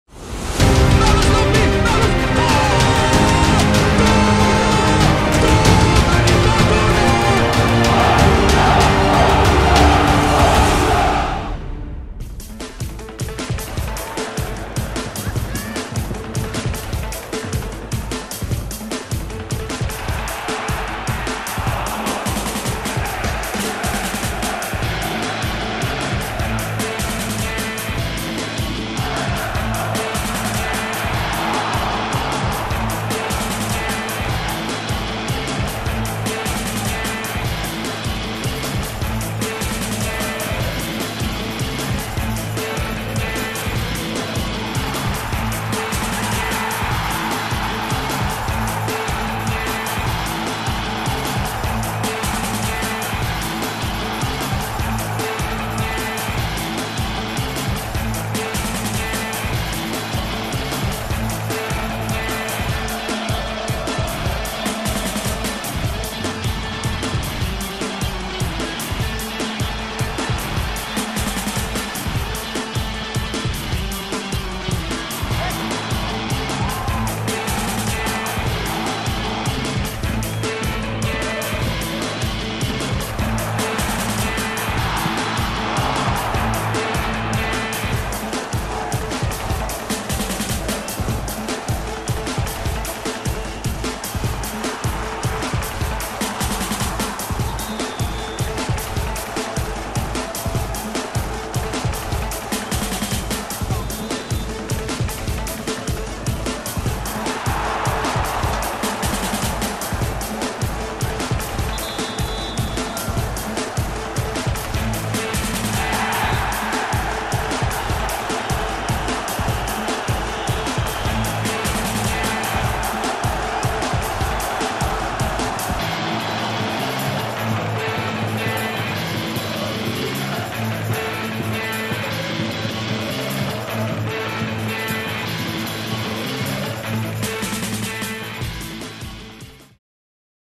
Et wor emol - Union vs. Fortuna
Andere Vorzeichen in der vergangenen Saison, aber mit einer engagierten und leidenschaftlichen Teamleistung konnten wir den Dreier von der Alten Försterei entführen.

Macht´s noch einmal Jungs und nehmt morgen etwas Zählbares aus Berlin mit!
P.s. Aber bitte ohne Gelb-Rot @Kaan Ayhan​ ;-)